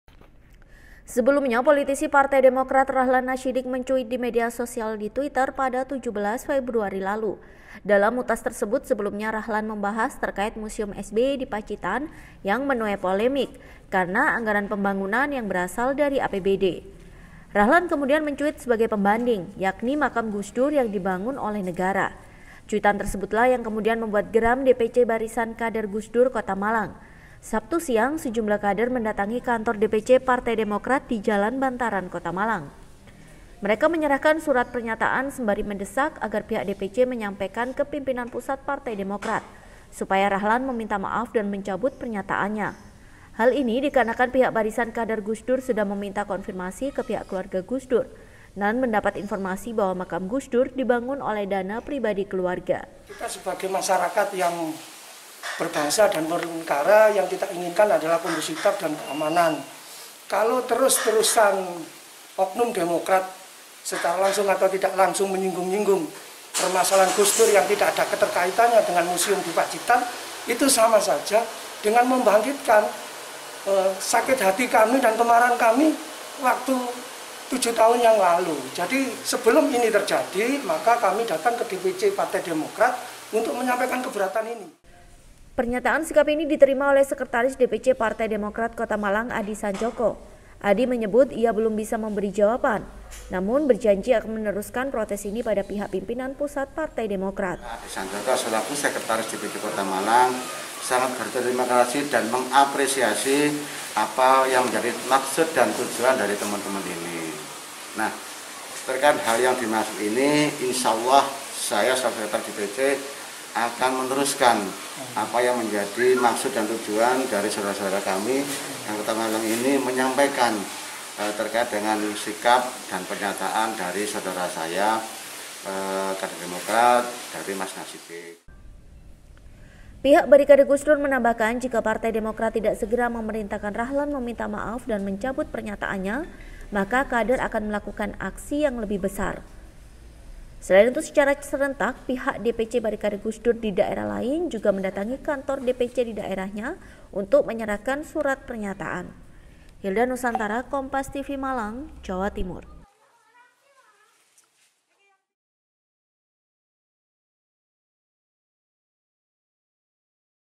Tak Terima Pernyataan Makam Gus Dur Dibangun Negara, Barikade Gus Dur Datangi DPC Partai Demokrat
MALANG-KOMPAS.TV-Tak terima pernyataan politisi Partai Demokrat Rachlan Nashidik, sejumlah anggota Barisan Kader  (Barikade) Gus Dur Kota Malang mendatangi kantor DPC Demokrat Kota Malang di Jalan Bantaran, Sabtu (20/02/2021).

Sebelumnya pada 17 Februari lalu Rachlan Nashidik mencuit di twitter dalam utas yang membahas polemik museum SBY yang anggaran pembangunan menggunakan APBD.

Rachlan mencuit sebagai pembanding, makam Gus Dur yang dibangun negara.

"Kami keberatan dengan apa yang disampaikan oleh Rachlan menyatakan bahwa itu dibangun oleh negara sama sekali tidak benar. Karena kita sudah konfirmasi ibu Yeni Wachid bahwa makam Gus Dur dibangun oleh swadaya keluarga" kata Ketua Barikade Gus Dur Kota Malang Dersi Hariono.

Mereka juga menyerahkan surat pernyataan yang diterima oleh sekretaris DPC Demokrat Kota Malang Adi Sancoko.

Adi menyebut akan menyampaikan protes ini ke pimpinan pusat.

"Kami tidak bisa memberikan putusan final. Semuanya ini ada prosedur tataran mekanisme dalam penanganan masalah nasional" jawab Adi.